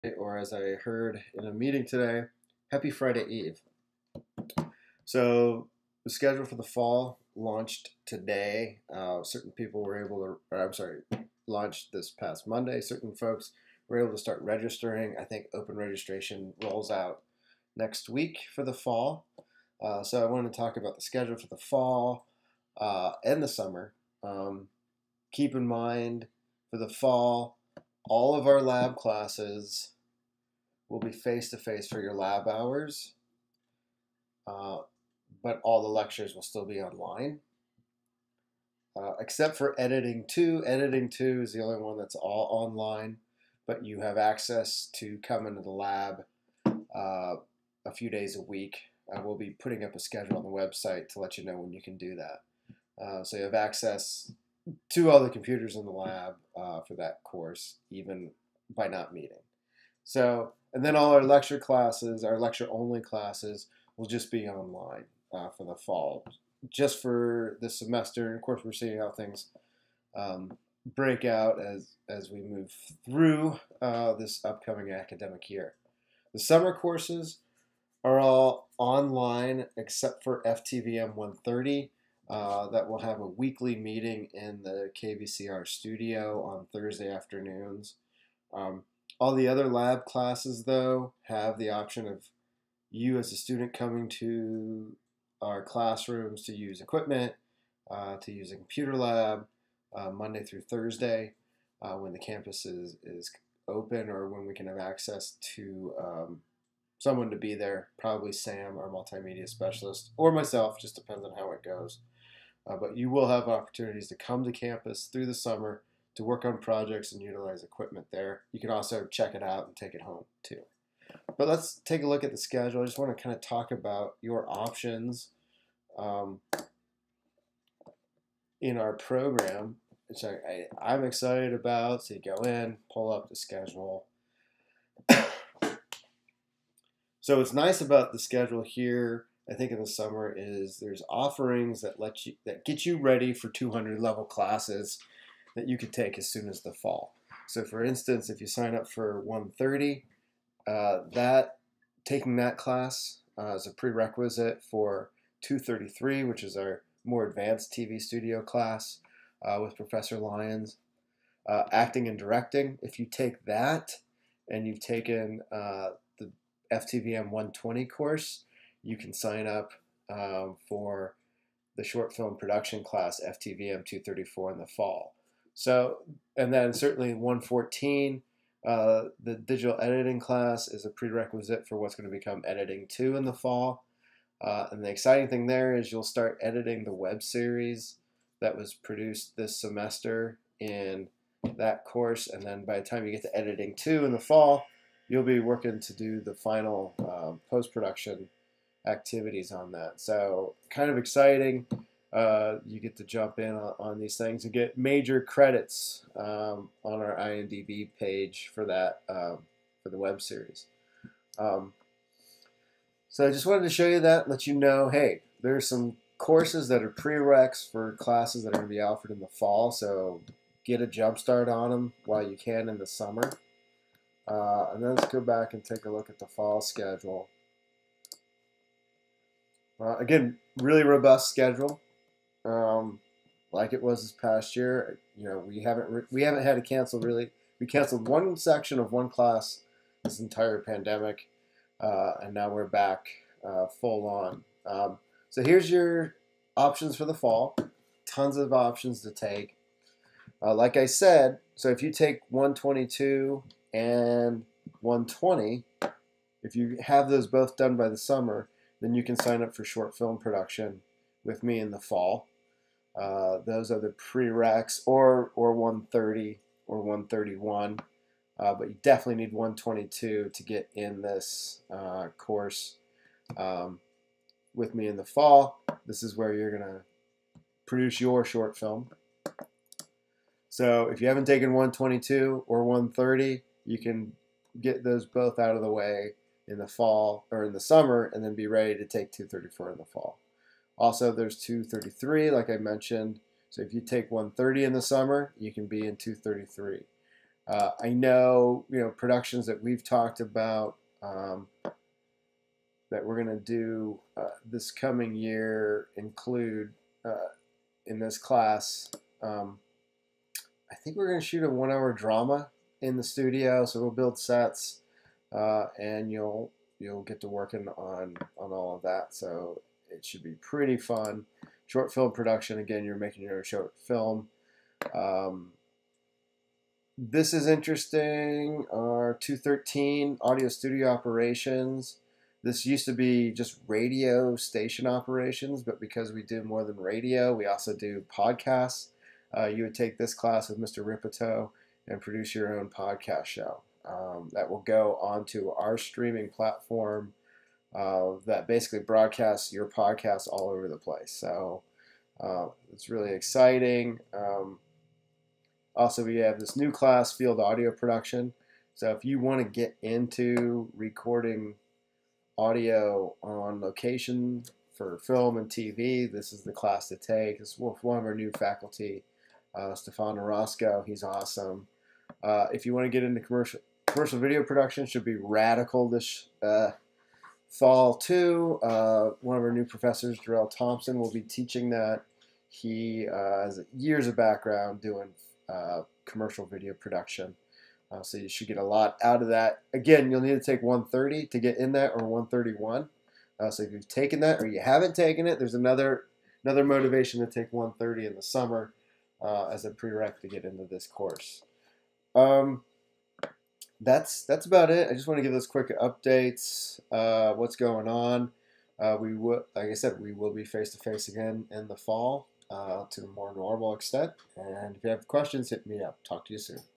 FTVM Schedule at San Bernardino Valley College for Summer and Fall 2021
Prof. Lucas talks about the schedules for summer and fall classes film,tv, and media courses at San Bernardino Valley College.